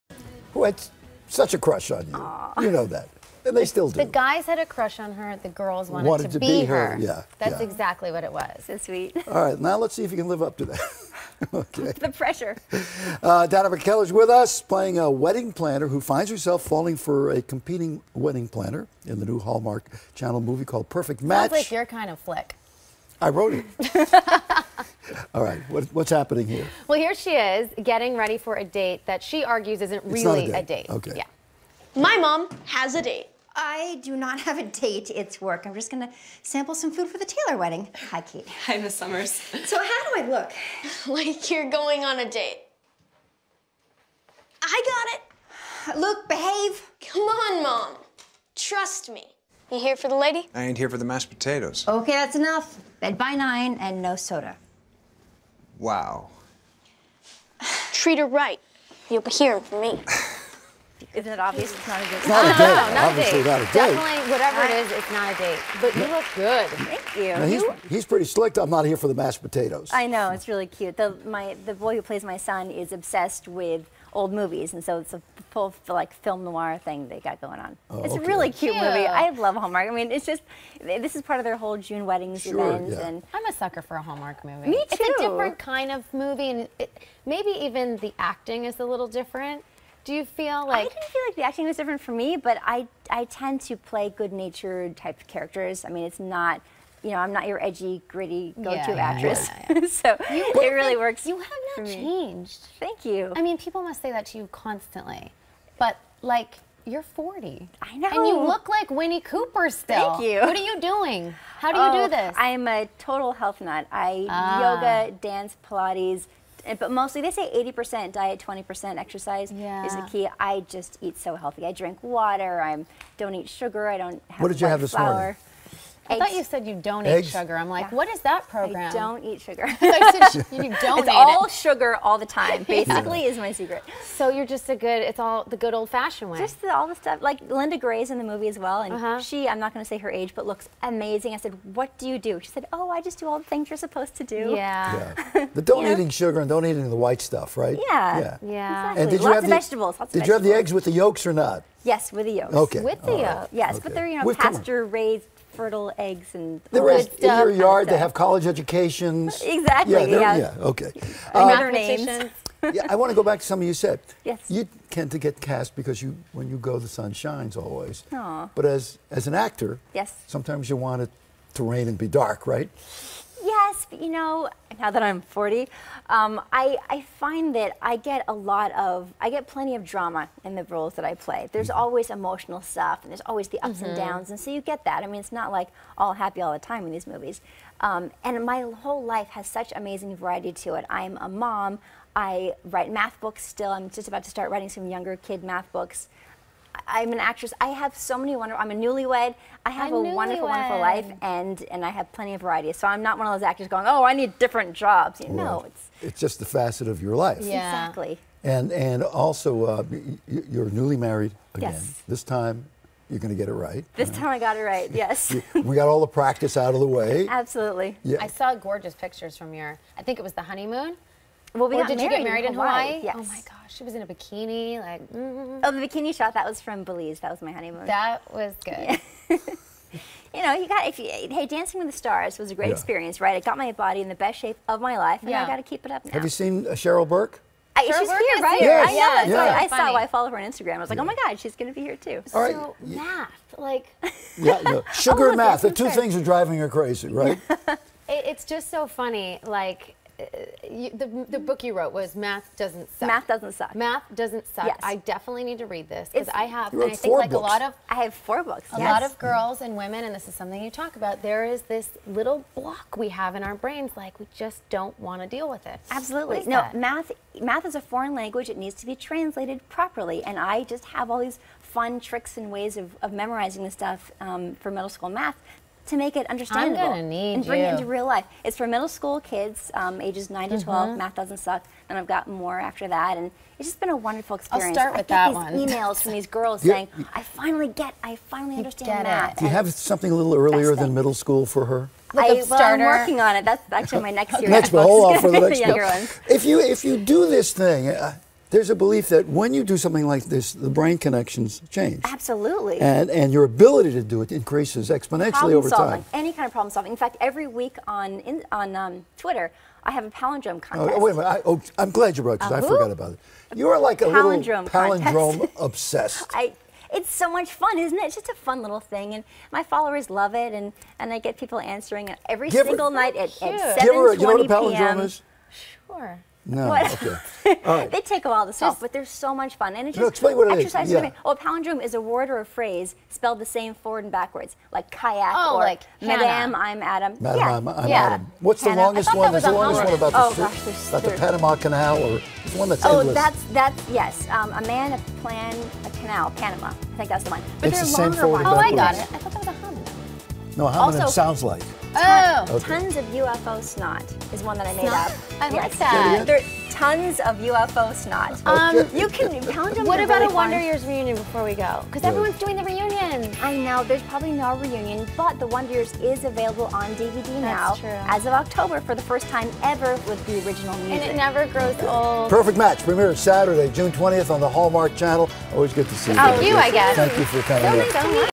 Danica McKellar Proves to The 'Perfect Match' In Hallmark's New Movie
Danica McKellar plays a wedding planner who finds herself falling for a competing wedding planner in the new Hallmark Channel movie ‘Perfect Match’.